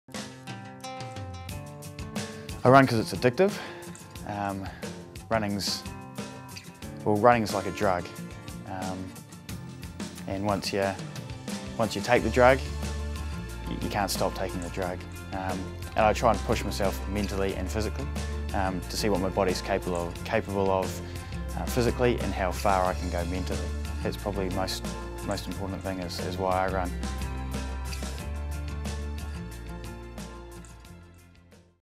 ASICS Why I Run - Chris
Chris, Runner, shares why he runs with ASICS